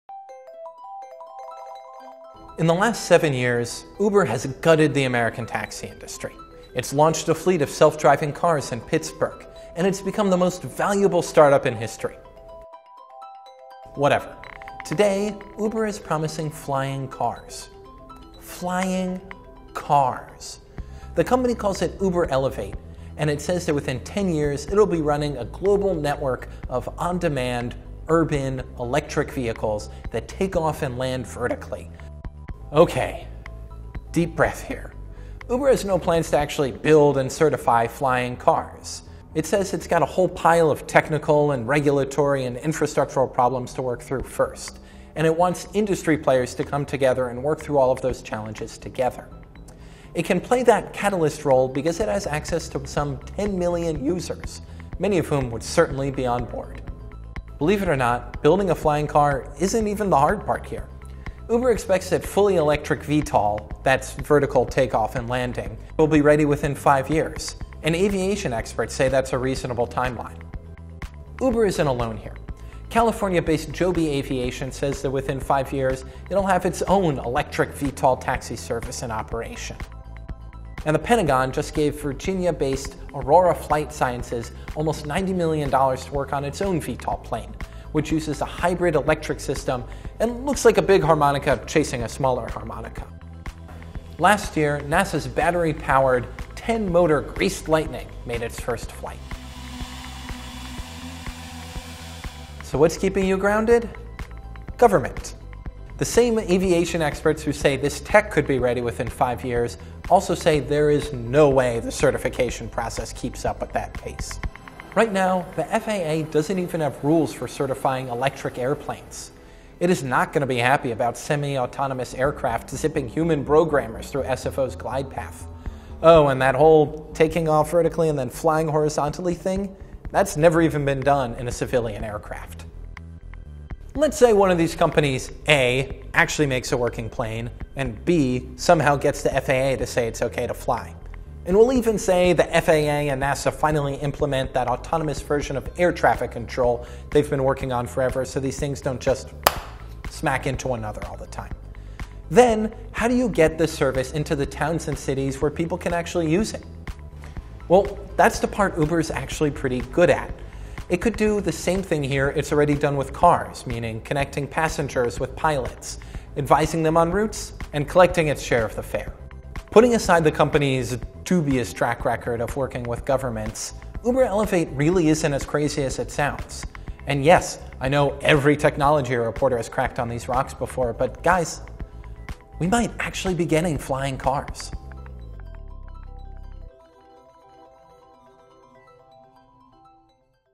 Guess What Uber's Promising Now: Flying Cars | WIRED
Forget self-driving cars, Uber has a new one for you... flying cars. The company calls it Uber Elevate and within a decade it’ll be a global network of on demand urban electric aircraft that take off and land vertically.